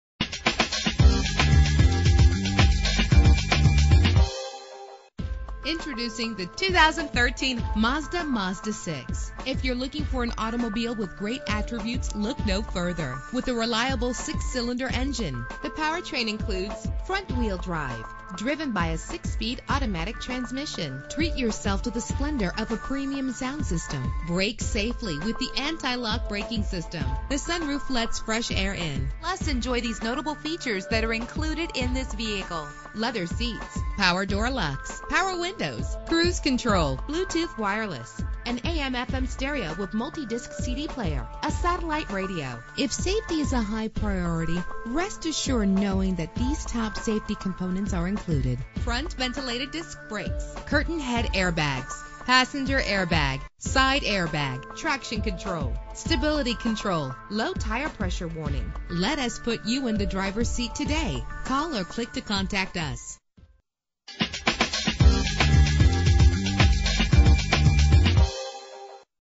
2013 Mazda MAZDA6 - Rensselar NY
Year: 2013
Make: Mazda
Model: MAZDA6
Trim: s Grand Touring
Engine: 3.7 liter 6 cylinder 24 valve
Transmission: 6-Speed Automatic
Color: Gray
Mileage: 4
Address: